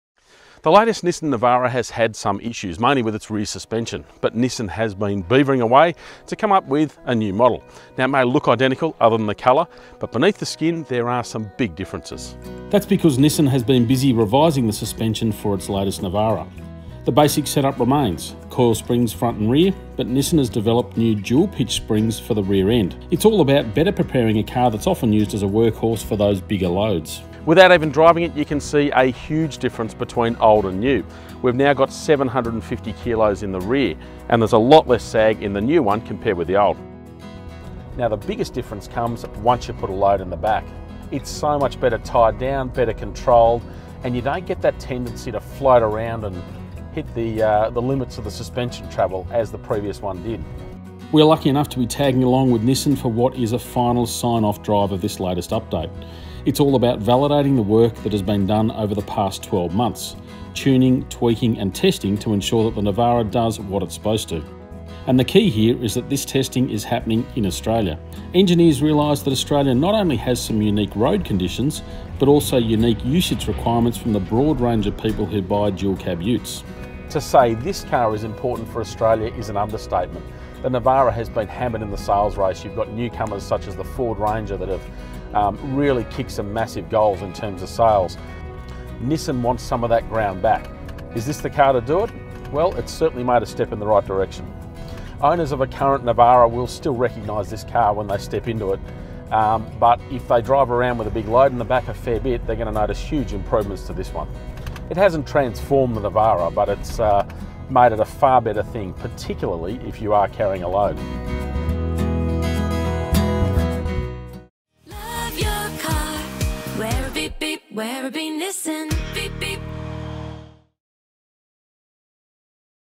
Nissan Navara - updated suspension.
Come for a ride in the new Nissan Navara and test the updated suspension.